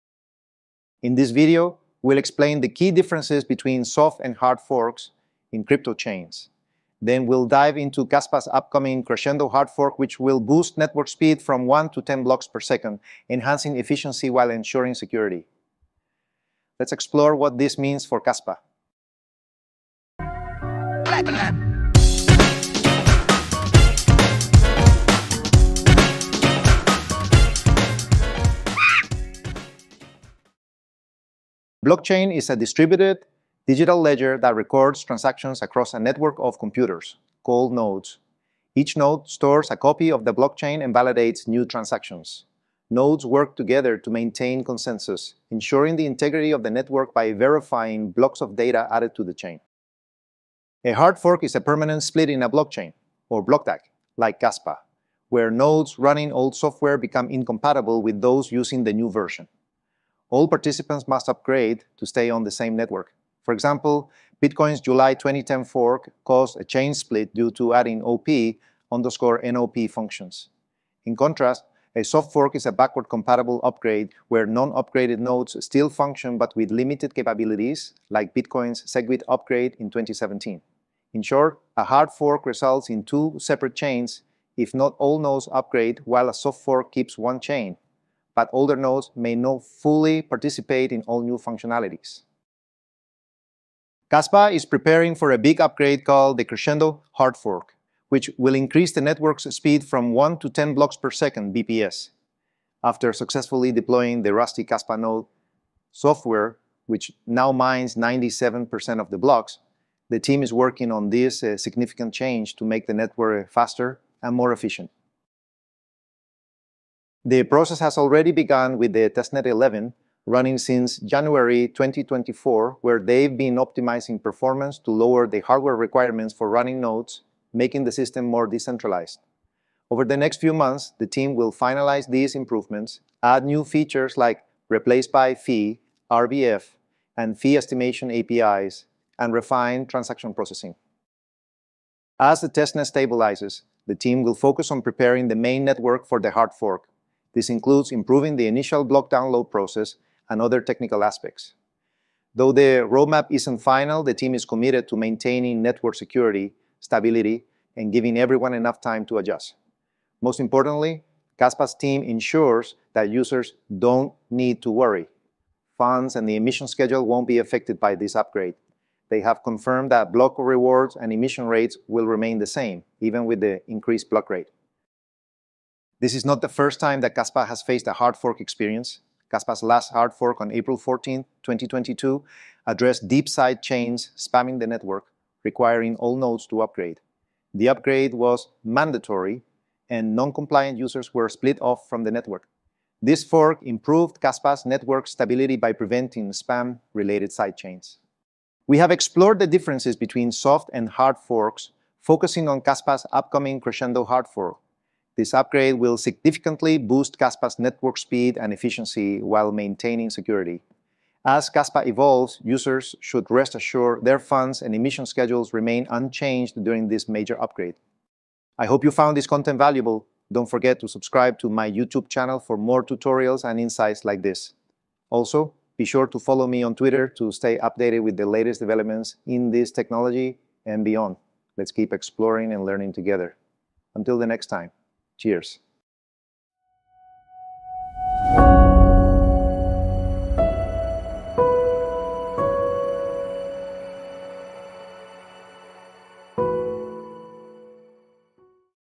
Soft Fork vs. Hard Fork: Understanding Kaspa’s Crescendo Upgrade
We break down the differences between soft and hard forks in crypto networks, explaining how each impacts blockchain operations. We then dive into Kaspa’s upcoming Crescendo hard fork, an exciting upgrade that will increase the network speed from 1 to 10 blocks per second, making transactions faster and more efficient without compromising security. With improvements already being tested on testnet 11, Kaspa’s team is preparing the main network for this major shift. Join us to learn what the Crescendo hard fork means for Kaspa and why it’s a significant step forward in blockchain technology!

Time codes:
00:00 - Intro
00:30 - Network Nodes
00:49 -  Hard Fork
01:01 - Bitcoin Examples
01:20 - Soft vs Hard Forks
01:32 - Kaspa Crescendo
02:40 - Keep Calm
02:55 - Previous Kaspa hard fork
03:18 - Summary
03:38 - Ending